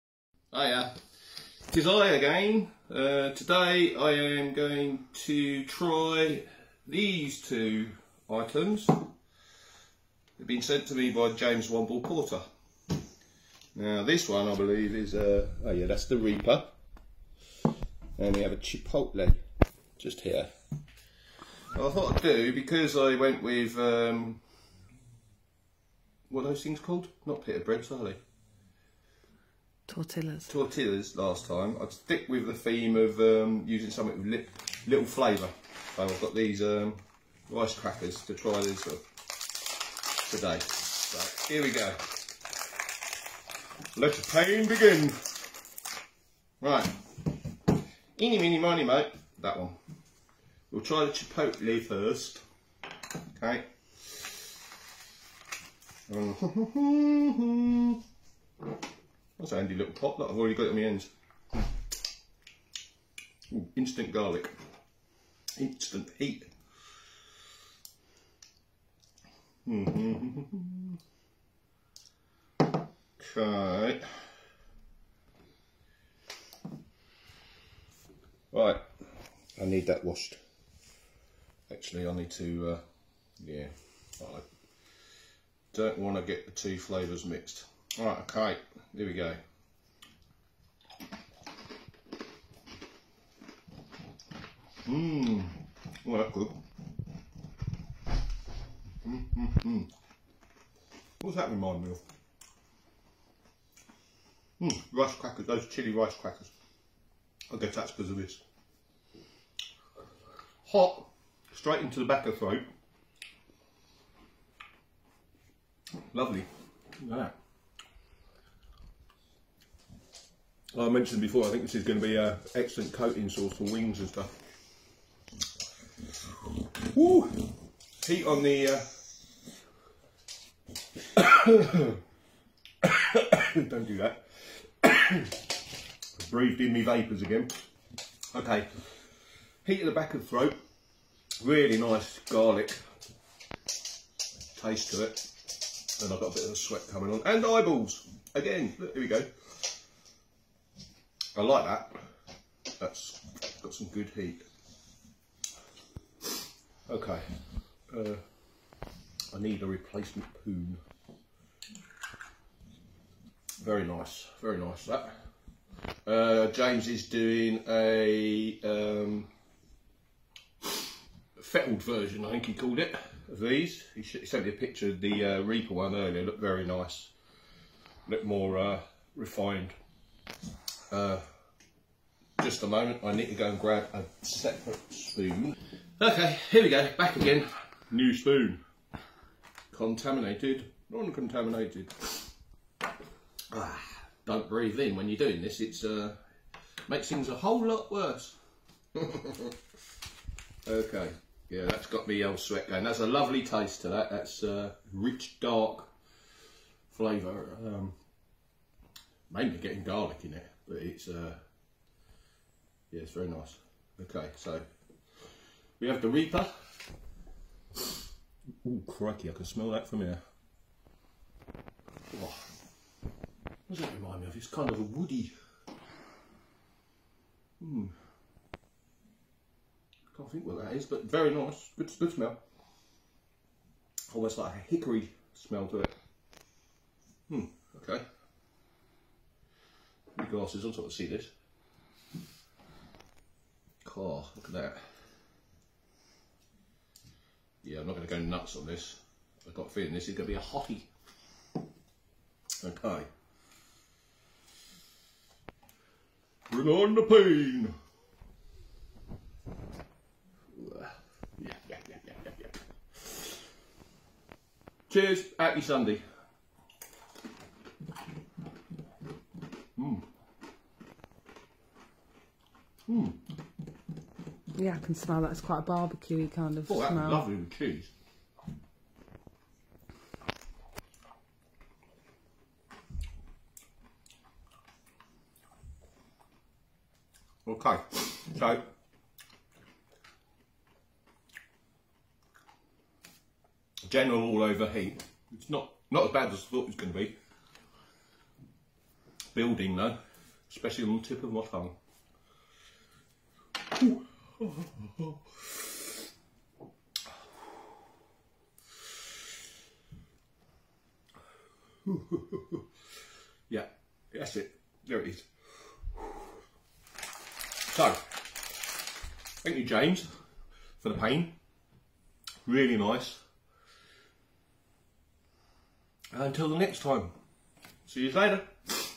Stuart Lutherborrow is here for a new review
Today he has some chilli pastes from James Womble Porter
Stuart starts with the chipotle paste then trys the reaper paste